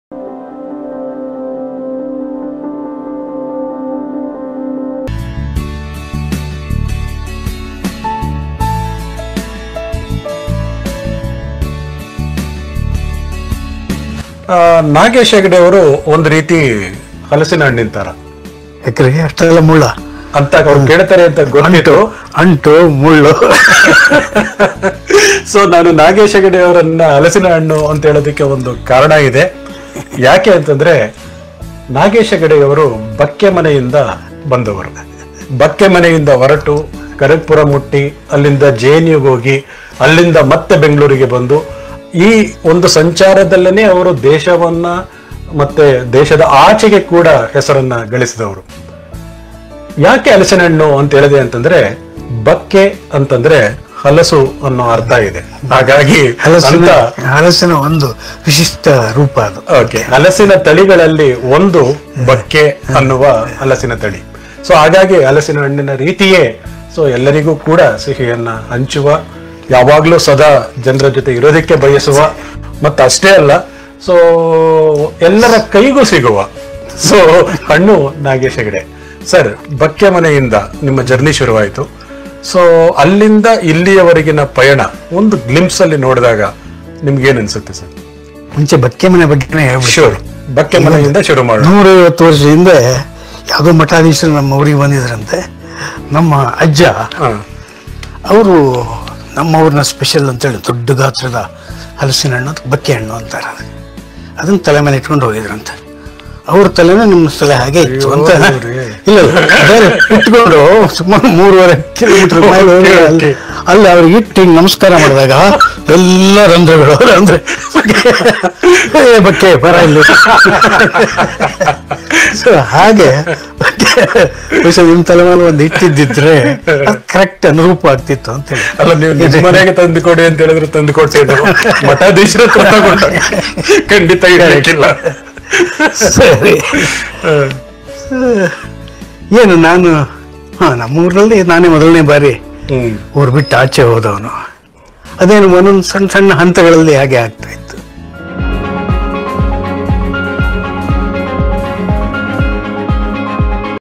Avadhi ‘Chalk Circle’ Special : chit chat With noted Journalist Nagesh Hegde -3
ನಾಗೇಶ್‌ ಹೆಗಡೆ ಝಲಕ್- 3 'ಅವಧಿ'ಯ 'ಚಾಕ್ ಸರ್ಕಲ್'ನಲ್ಲಿನ ಮಾತುಕತೆಯ ಆಯ್ದ ಭಾಗ